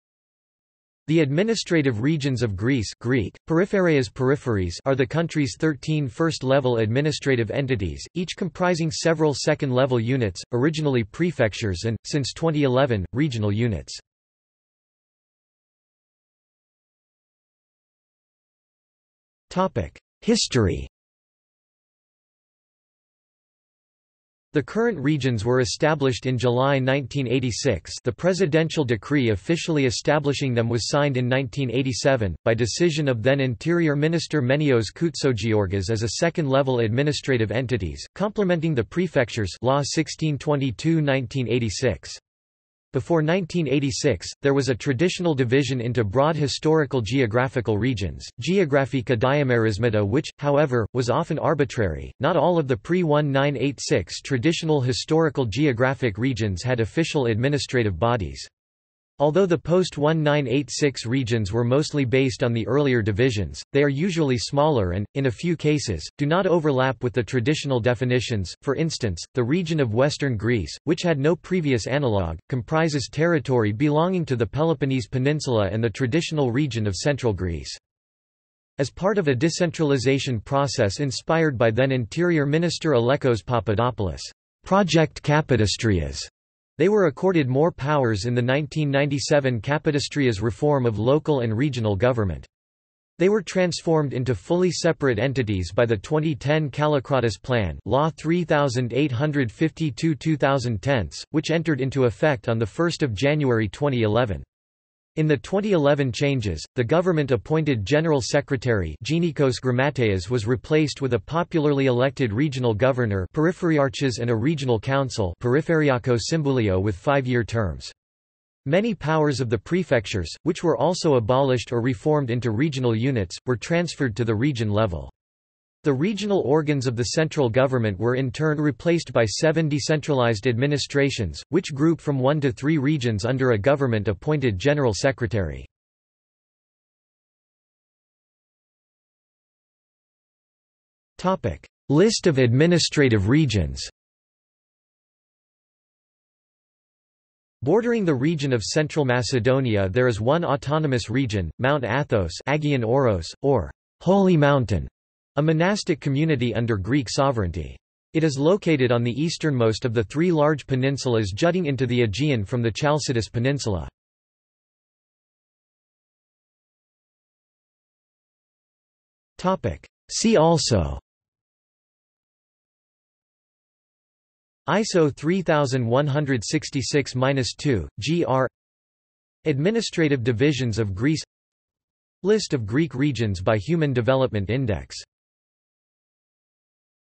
Periphery (country subdivision) | Wikipedia audio article
This is an audio version of the Wikipedia Article:
Periphery (country subdivision)

00:00:15 1 History
00:02:17 2 List of administrative regions
00:02:45 3 See also

- Socrates

SUMMARY
The administrative regions of Greece (Greek: περιφέρειες, peripheries) are the country's thirteen first-level administrative entities, each comprising several second-level units, originally prefectures and, since 2011, regional units.